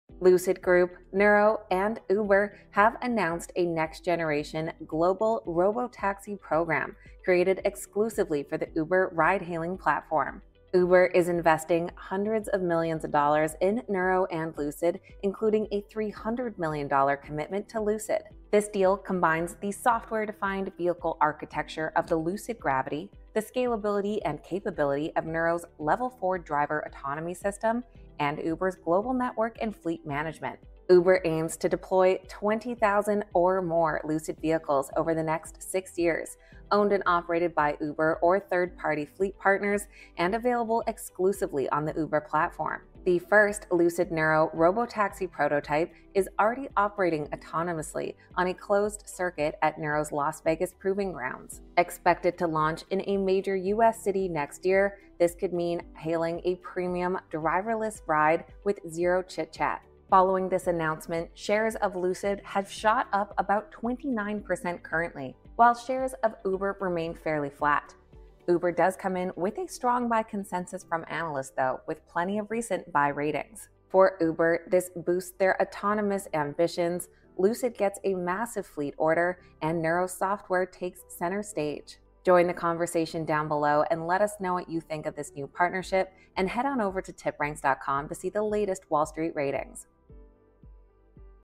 Lucid, Nuro & Uber: The Future of Driverless Ride Hailing
Big news in the EV and autonomous driving world! Uber is teaming up with Lucid Motors Group and Nuro for a premium global robotaxi program, investing multi-hundred-millions—including $300M in Lucid—to deploy 20,000+ Lucid Gravity vehicles with Nuro's Level 4 autonomy over the next six years.

The first prototype is already zipping around autonomously in Las Vegas on a closed circuit, and launches could hit a major U.S. city next year!

Shares of LCID have gained 29% on the announcement, while UBER shares remain fairly flat. See how Wall Street analysts weigh in on TipRanks

This partnership could supercharge the robotaxi revolution and boost these stocks. What the video and let us know: Bullish on UBER, LCID, or both?